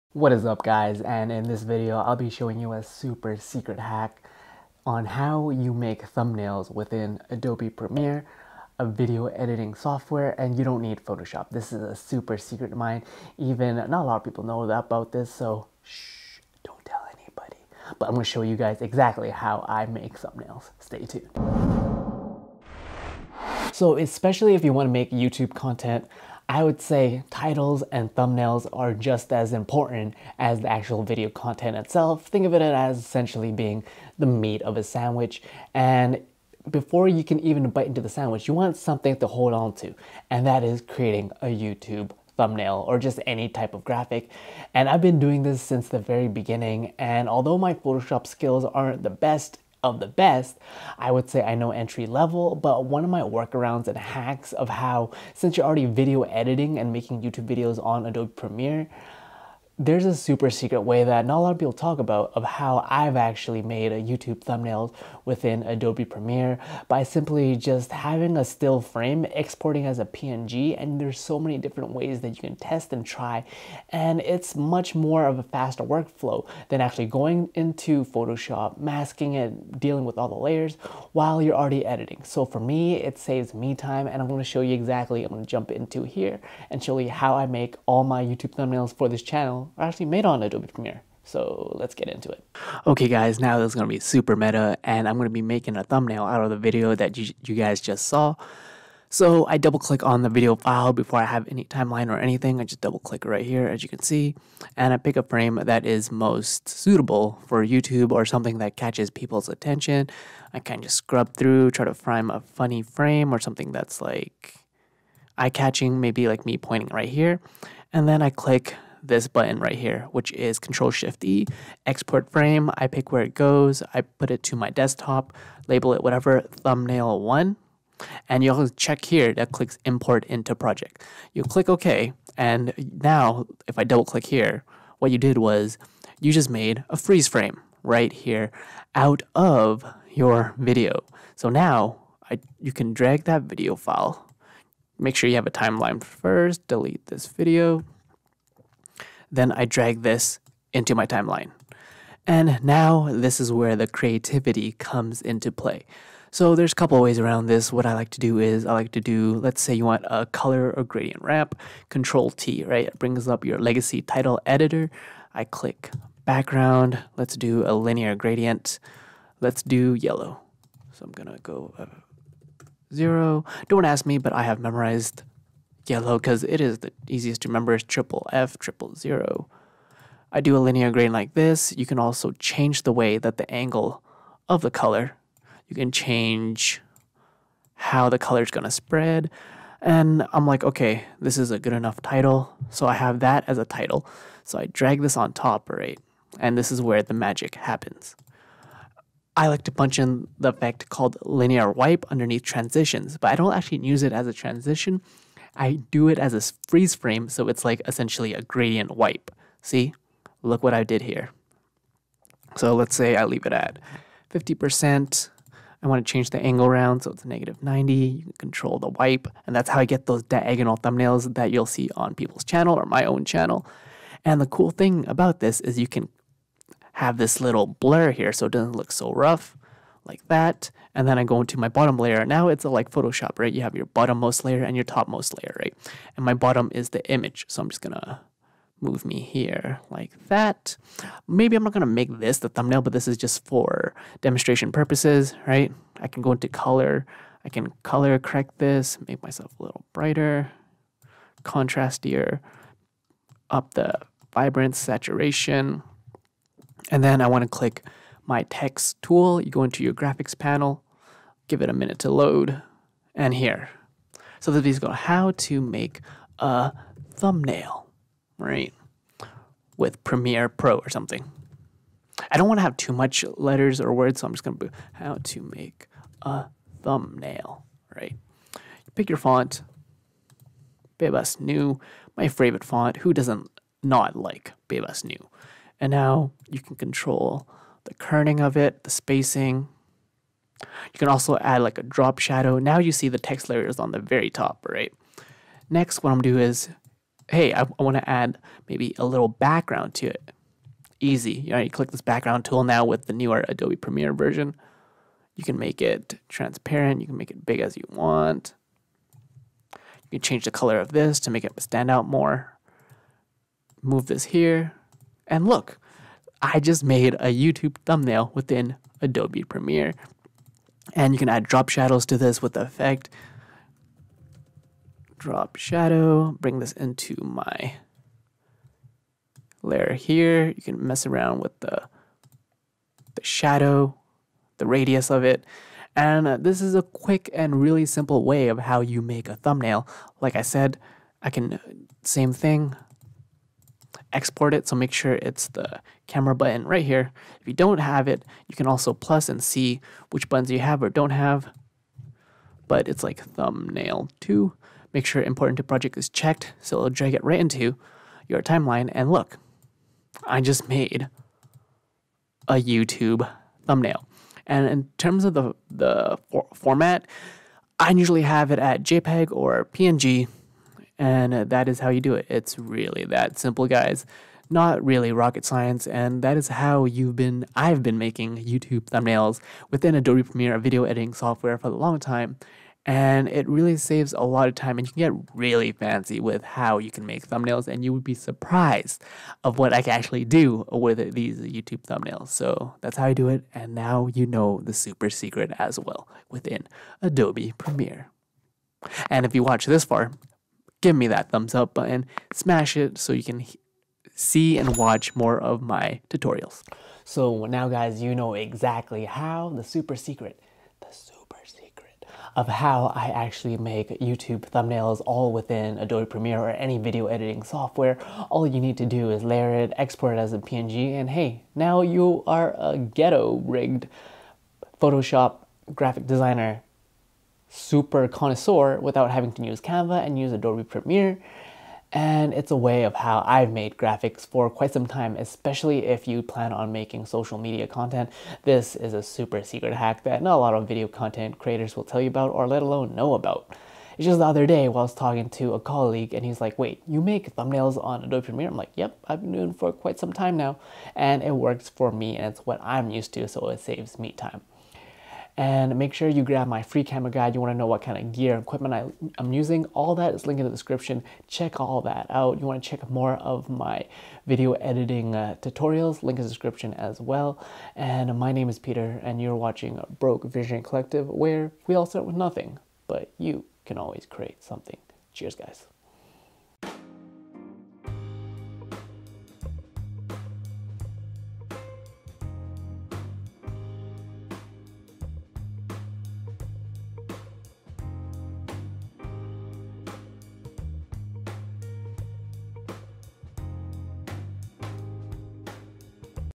Creating a CUSTOM YouTube Thumbnail in Premiere Pro?? (TUTORIAL)
How do you create a custom YouTube thumbnail in Premiere Pro? In this video I show you the step by step tutorial on how I've been creating all my YouTube thumbnails exclusively here.

🎥Camera I use: